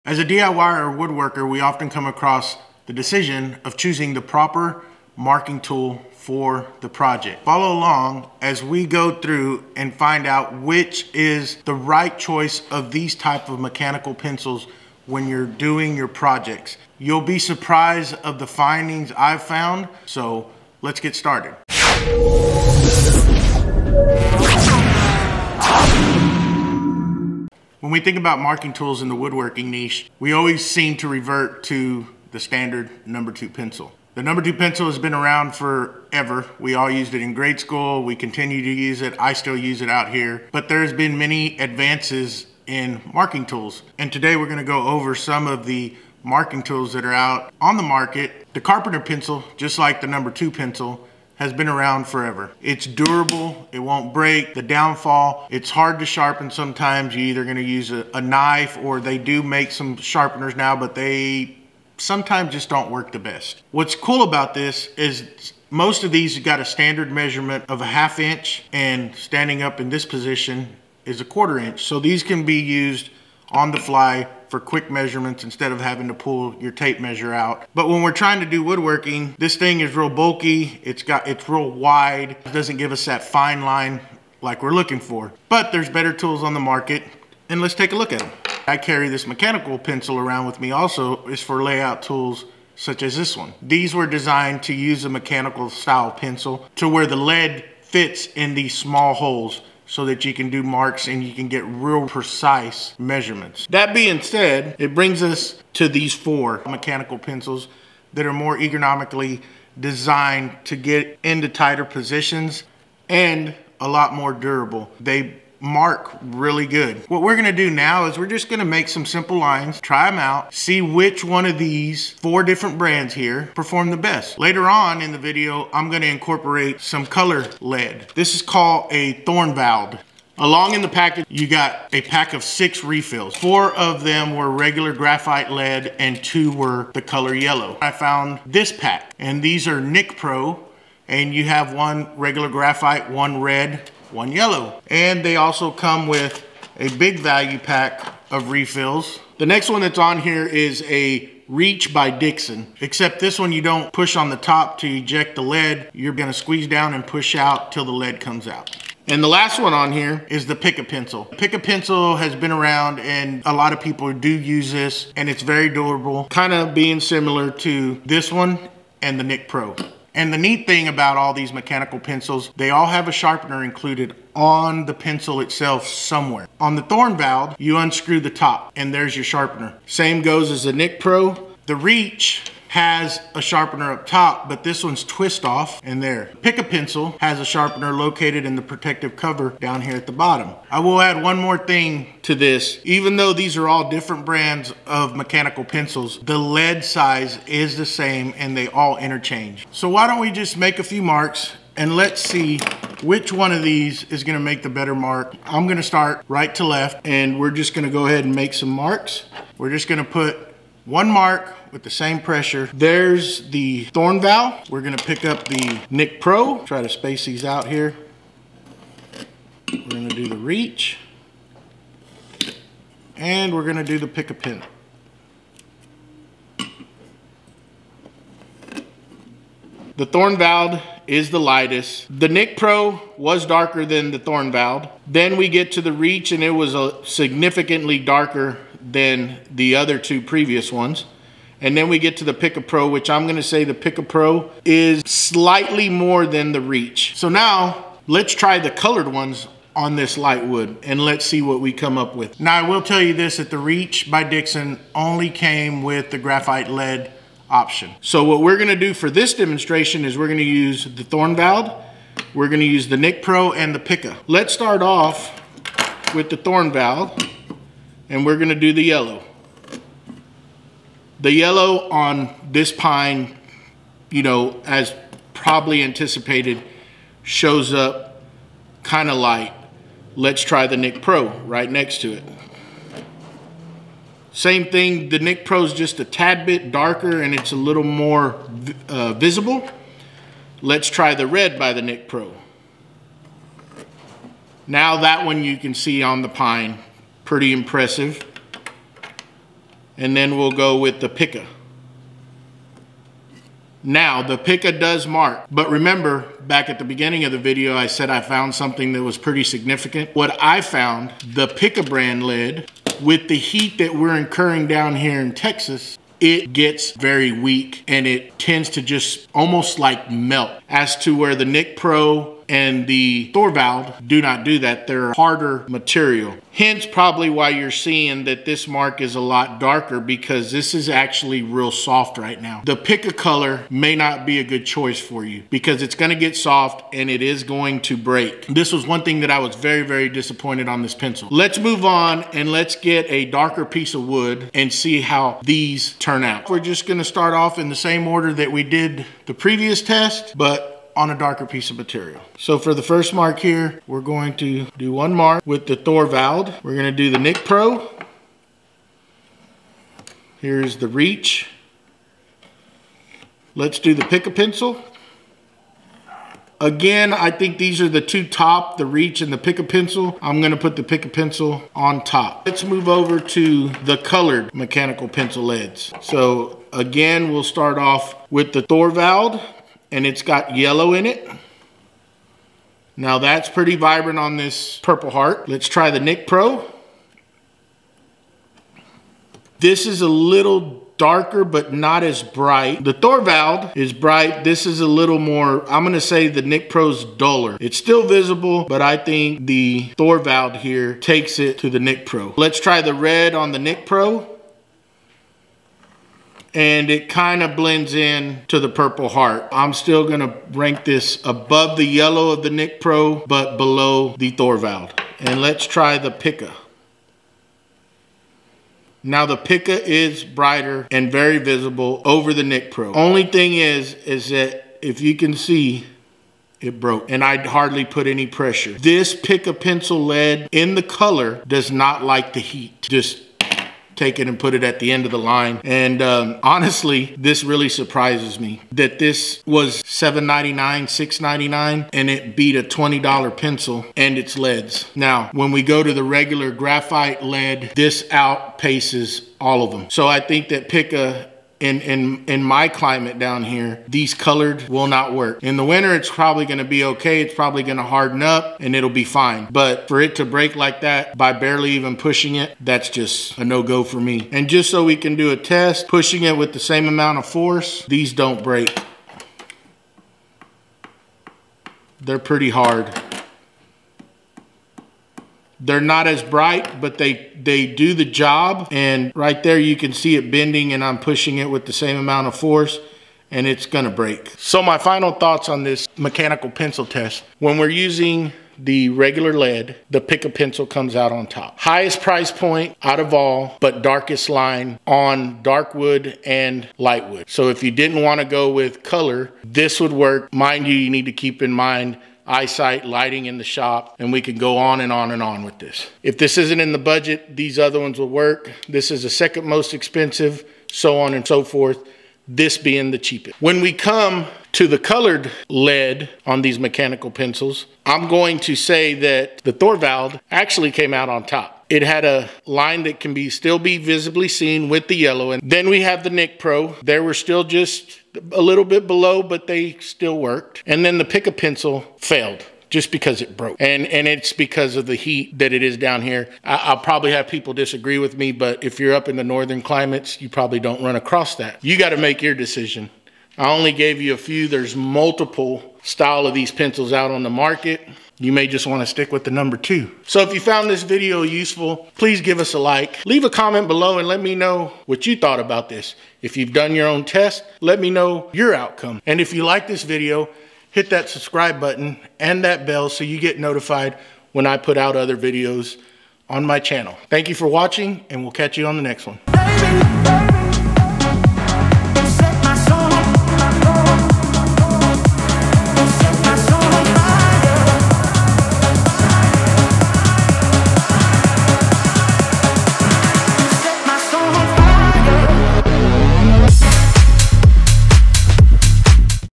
Mechanical carpenter pencil shootout!
This video is a review of the Pica, Thorvald, Dixon and NicPro mechanical pencils. The woodworking mechanical pencils are tested and compared to reveal which brand comes out on top. The shootout also reveals an interesting problem with the pica dry longlife automatic marking pencil colored pencil lead.